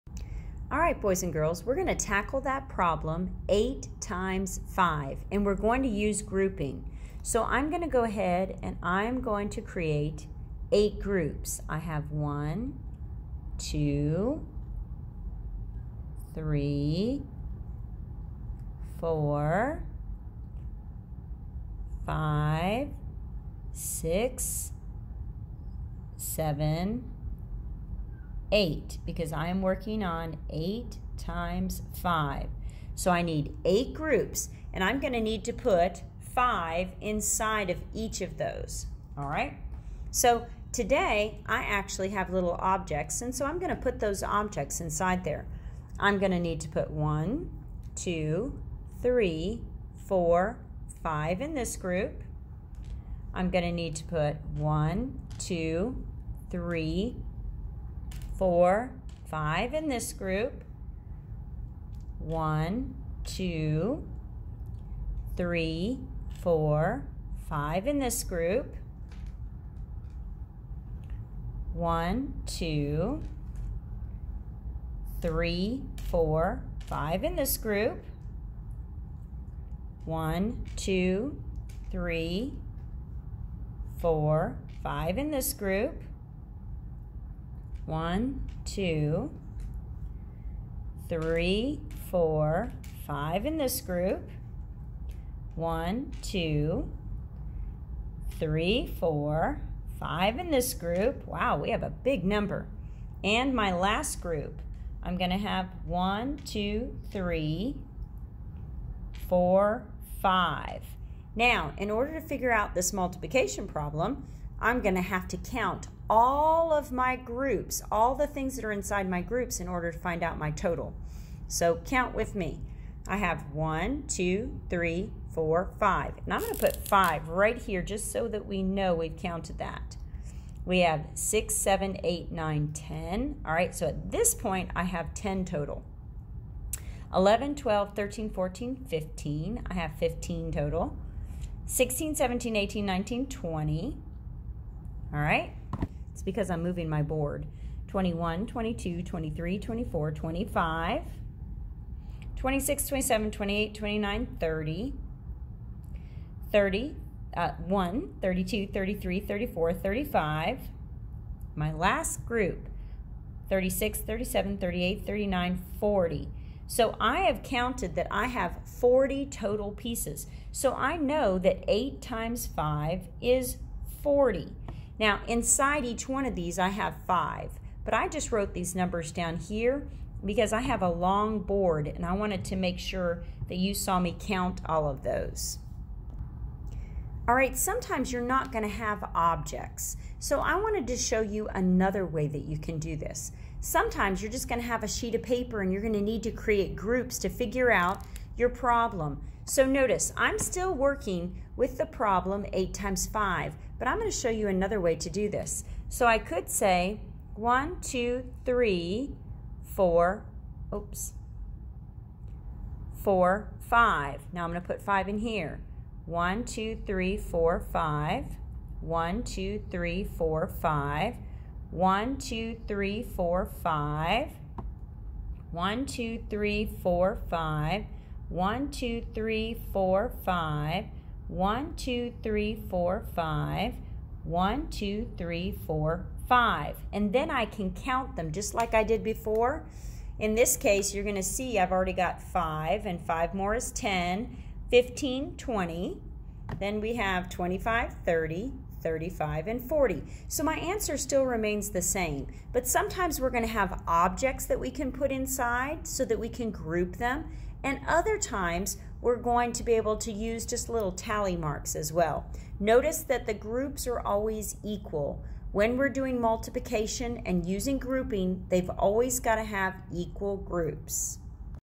Making Groups to solve 8x5
students will watch an example of how to use the grouping strategy to find the answer to 8x5.